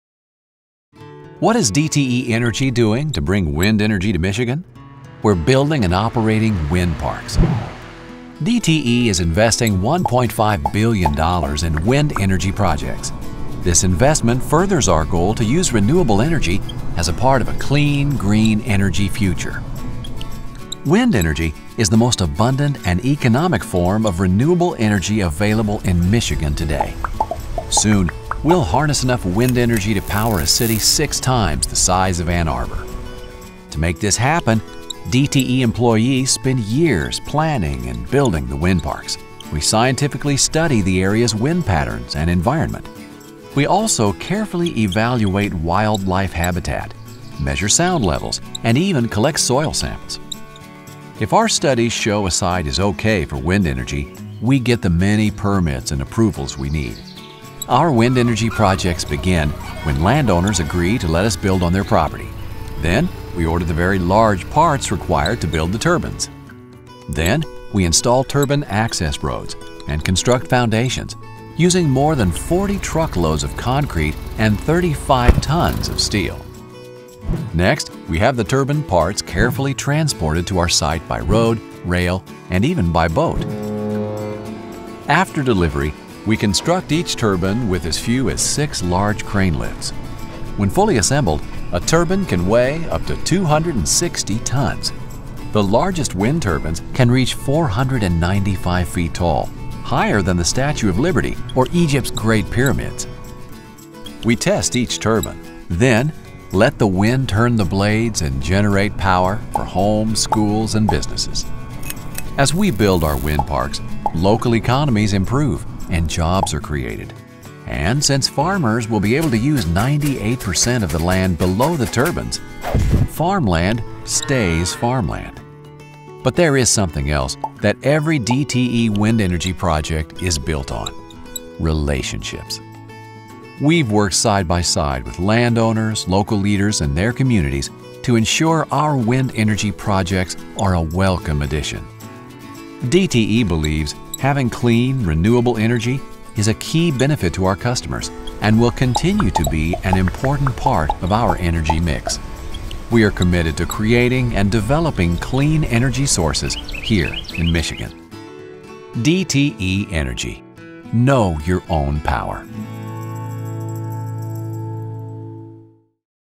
What's DTE Energy Doing to Bring Wind Energy to Michigan?
DTE Energy believes that wind energy is a vital part of the energy mix to meet Michigan's future energy needs. Harnessing the wind offers many benefits to our customers, the environment, the land owners, the local community, and our stakeholders.